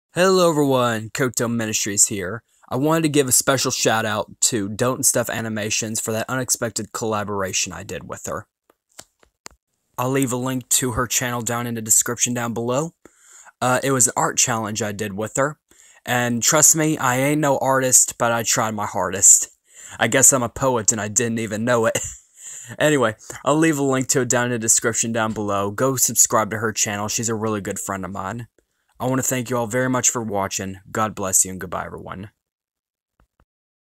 Thank you DOTEandStuff animations
She is a very good friend of mine I hope you enjoy.

 I know that art I did wasn't very good but trust me if I had more time it would look better.